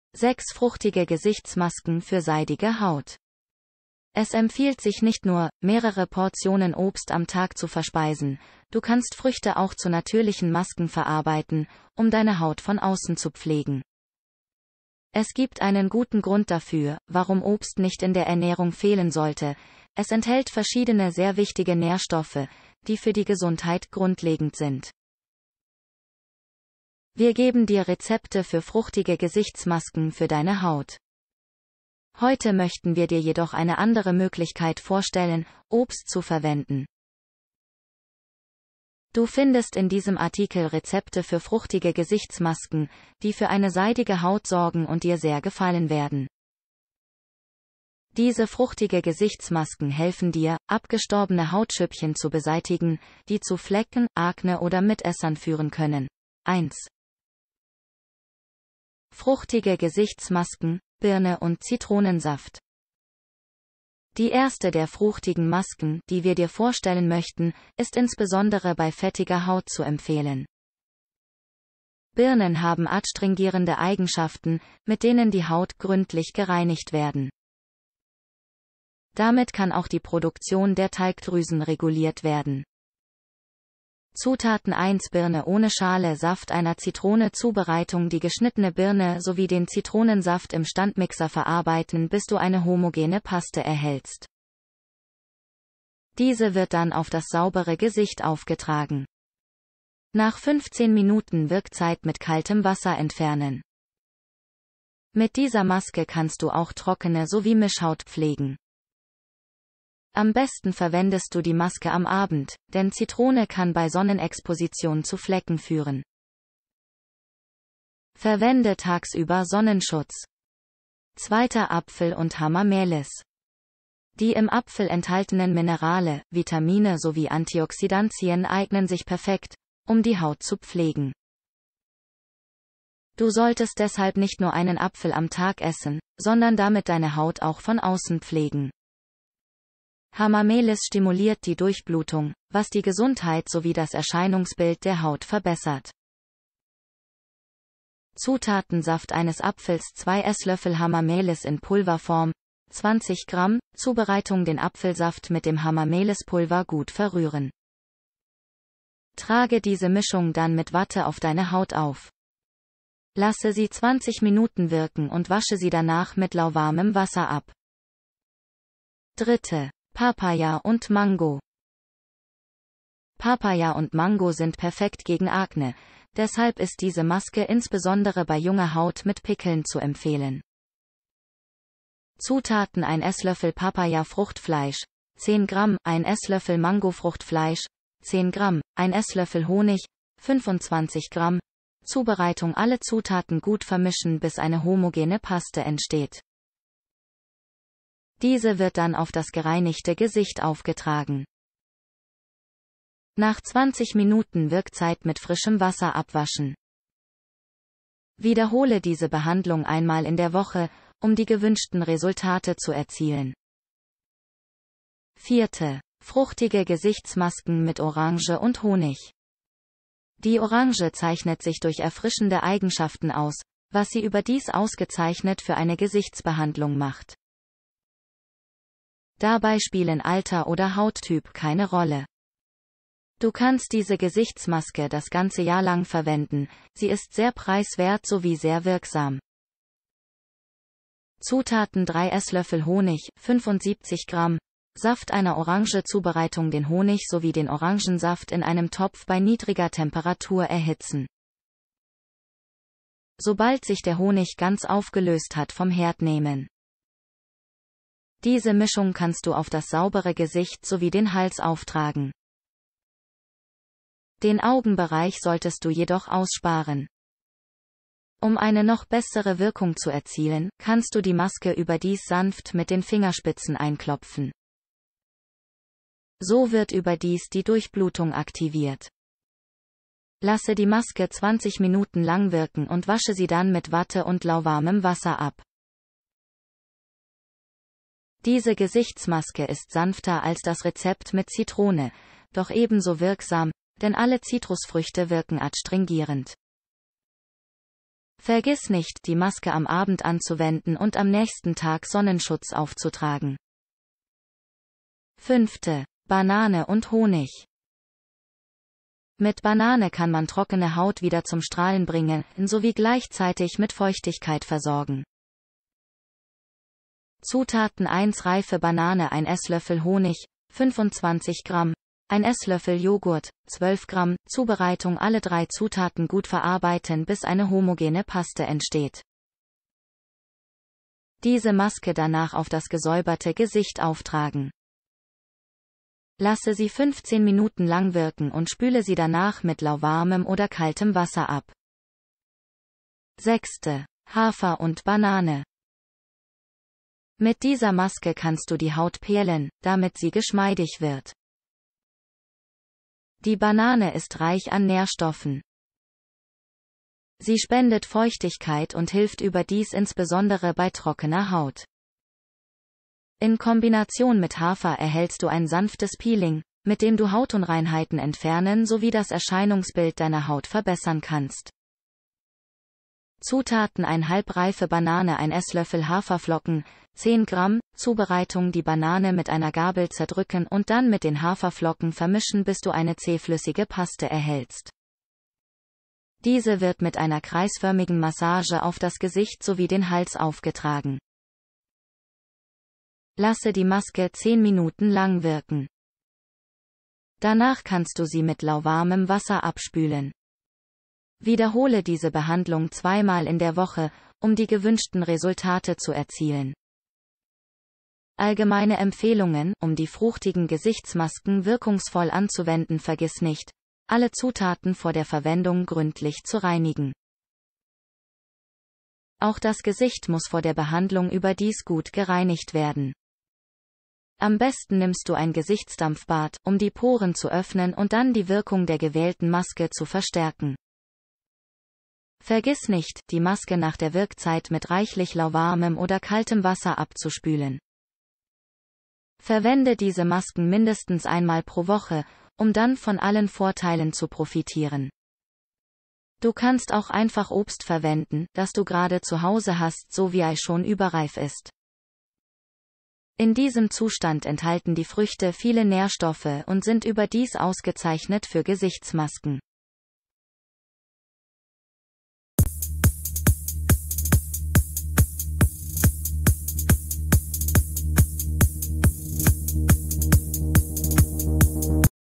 6 fruchtige Gesichtsmasken für seidige Haut
6 fruchtige Gesichtsmasken für seidige Haut

6 fruchtige Gesichtsmasken für seidige Haut
Es empfiehlt sich nicht nur, mehrere Portionen Obst am Tag zu verspeisen, du kannst Früchte auch zu natürlichen Masken verarbeiten, um deine Haut von außen zu pflegen.
Es gibt einen guten Grund dafür, warum Obst nicht in der Ernährung fehlen sollte: Es enthält verschiedene sehr wichtige Nährstoffe, die für die Gesundheit grundlegend sind. Wir geben dir Rezepte für fruchtige Gesichtsmasken für deine Haut.
Heute möchten wir dir jedoch eine andere Möglichkeit vorstellen, Obst zu verwenden. Du findest in diesem Artikel Rezepte für fruchtige Gesichtsmasken, die für eine seidige Haut sorgen und dir sehr gefallen werden.
Diese fruchtige Gesichtsmasken helfen dir, abgestorbene Hautschüppchen zu beseitigen, die zu Flecken, Akne oder Mitessern führen können.1. Fruchtige Gesichtsmasken: Birne und Zitronensaft
Die erste der fruchtigen Masken, die wir dir vorstellen möchten, ist insbesondere bei fettiger Haut zu empfehlen.
Birnen haben adstringierende Eigenschaften, mit denen die Haut gründlich gereinigt werden. Damit kann auch die Produktion der Talgdrüsen reguliert werden.
Zutaten 1 Birne ohne Schale Saft einer Zitrone Zubereitung Die geschnittene Birne sowie den Zitronensaft im Standmixer verarbeiten bis du eine homogene Paste erhältst. Diese wird dann auf das saubere Gesicht aufgetragen. Nach 15 Minuten Wirkzeit mit kaltem Wasser entfernen.
Mit dieser Maske kannst du auch trockene sowie Mischhaut pflegen.
Am besten verwendest du die Maske am Abend, denn Zitrone kann bei Sonnenexposition zu Flecken führen. Verwende tagsüber Sonnenschutz!
2. Apfel und Hamamelis
Die im Apfel enthaltenen Minerale, Vitamine sowie Antioxidantien eignen sich perfekt, um die Haut zu pflegen.
Du solltest deshalb nicht nur einen Apfel am Tag essen, sondern damit deine Haut auch von außen pflegen.
Hamamelis stimuliert die Durchblutung, was die Gesundheit sowie das Erscheinungsbild der Haut verbessert.
Zutaten Saft eines Apfels 2 Esslöffel Hamamelis in Pulverform (20 g) Zubereitung Den Apfelsaft mit dem Hamamelispulver gut verrühren. Trage diese Mischung dann mit Watte auf deine Haut auf. Lasse sie 20 Minuten wirken und wasche sie danach mit lauwarmem Wasser ab. 3. Papaya und Mango
Papaya und Mango sind perfekt gegen Akne, deshalb ist diese Maske insbesondere bei junger Haut mit Pickeln zu empfehlen.
Zutaten 1 Esslöffel Papayafruchtfleisch (10 g) ein Esslöffel Mangofruchtfleisch (10 g) 1 Esslöffel Honig (25 g) Zubereitung Alle Zutaten gut vermischen bis eine homogene Paste entsteht. Diese wird dann auf das gereinigte Gesicht aufgetragen. Nach 20 Minuten Wirkzeit mit frischem Wasser abwaschen. Wiederhole diese Behandlung einmal in der Woche, um die gewünschten Resultate zu erzielen. 4. Fruchtige Gesichtsmasken mit Orange und Honig

Die Orange zeichnet sich durch erfrischende Eigenschaften aus, was sie überdies ausgezeichnet für eine Gesichtsbehandlung macht. Dabei spielen Alter oder Hauttyp keine Rolle.
Du kannst diese Gesichtsmaske das ganze Jahr lang verwenden, sie ist sehr preiswert sowie sehr wirksam.
Zutaten 3 Esslöffel Honig (75 g) Saft einer Orange Zubereitung Den Honig sowie den Orangensaft in einem Topf bei niedriger Temperatur erhitzen. Sobald sich der Honig ganz aufgelöst hat vom Herd nehmen. Diese Mischung kannst du auf das saubere Gesicht sowie den Hals auftragen. Den Augenbereich solltest du jedoch aussparen. Um eine noch besssere Wirkung zu erzielen, kannst du die Maske überdies sanft mit den Fingerspitzen einklopfen. So wird überdies die Durchblutung aktiviert. Lasse die Maske 20 Minuten lang wirken und wasche sie dann mit Watte und lauwarmem Wasser ab.
Diese Gesichtsmaske ist sanfter als das Rezept mit Zitrone, doch ebenso wirksam, denn alle Zitrusfrüchte wirken adstringierend.
Vergiss nicht, die Maske am Abend anzuwenden und am nächsten Tag Sonnenschutz aufzutragen.
5. Banane und Honig
Mit Banane kann man trockene Haut wieder zum Strahlen bringen sowie gleichzeitig mit Feuchtigkeit versorgen.
Zutaten 1 reife Banane ein Esslöffel Honig (25 g) 1 Esslöffel Joghurt (12 g) Zubereitung Alle drei Zutaten gut verarbeiten bis eine homogene Paste entsteht. Diese Maske danach auf das gesäuberte Gesicht auftragen. Lasse sie 15 Minuten lang wirken und spüle sie danach mit lauwarmem oder kaltem Wasser ab. 6. Hafer und Banane
Mit dieser Maske kannst du die Haut peelen, damit sie geschmeidig wird.
Die Banane ist reich an Nährstoffen. Sie spendet Feuchtigkeit und hilft überdies insbesondere bei trockener Haut.
In Kombination mit Hafer erhältst du ein sanftes Peeling, mit dem du Hautunreinheiten entfernen sowie das Erscheinungsbild deiner Haut verbessern kannst.
Zutaten ½ reife Banane 1 Esslöffel Haferflocken (10 g) Zubereitung Die Banane mit einer Gabel zer